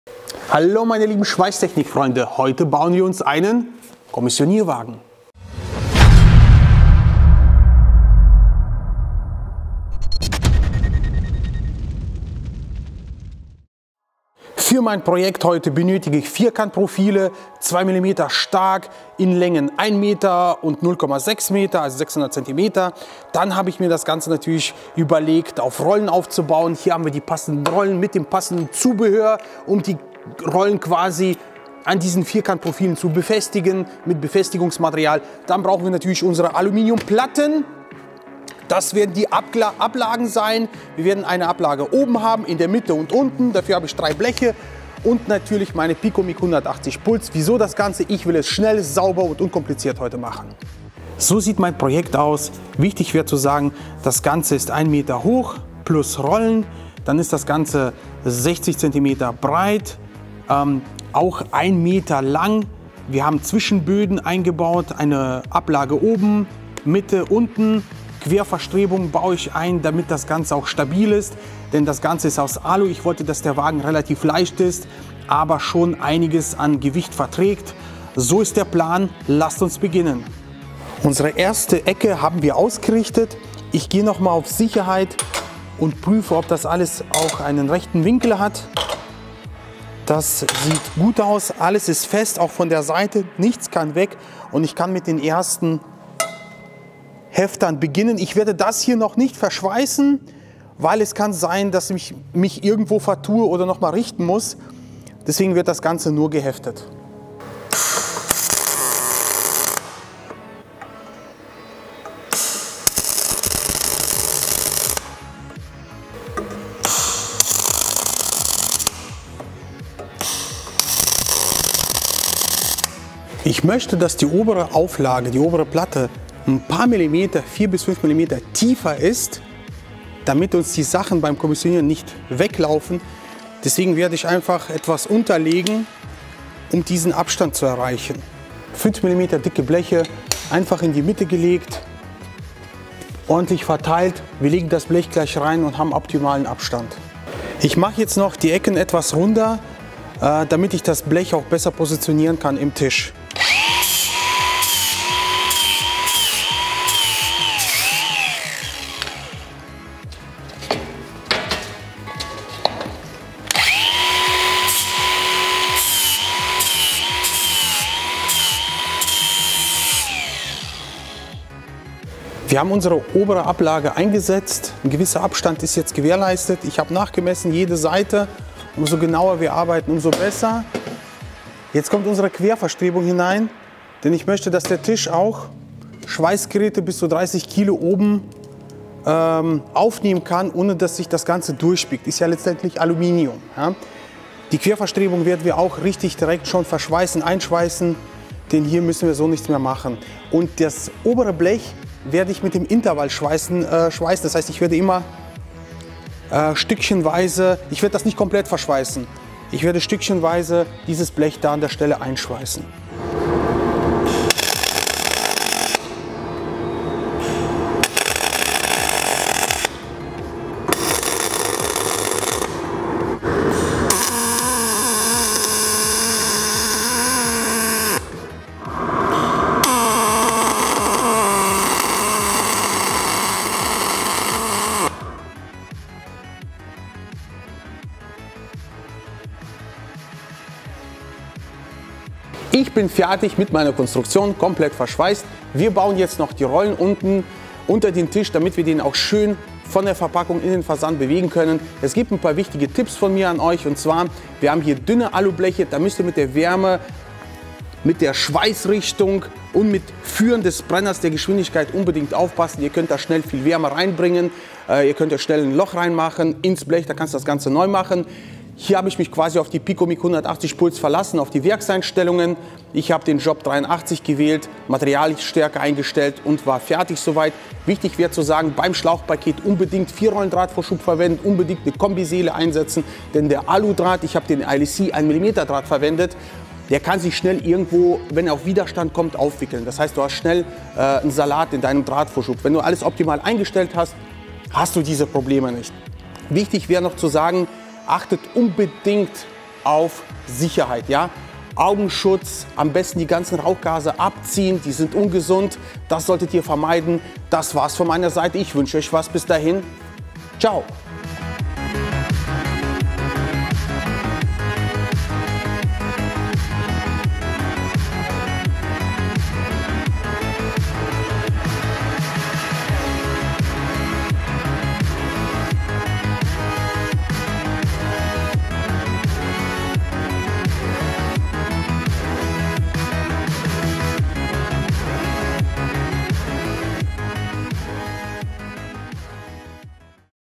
Aluminium Vierkantrohr Transportwagen schnell und einfach schweißen - mig pulsen mit ewm Picomig
Hallo meine lieben Schweißtechnikfreunde!

HDB Schweiß Shop - Alles rund ums Thema Schweißen und mehr!

xxProduktplazierungxx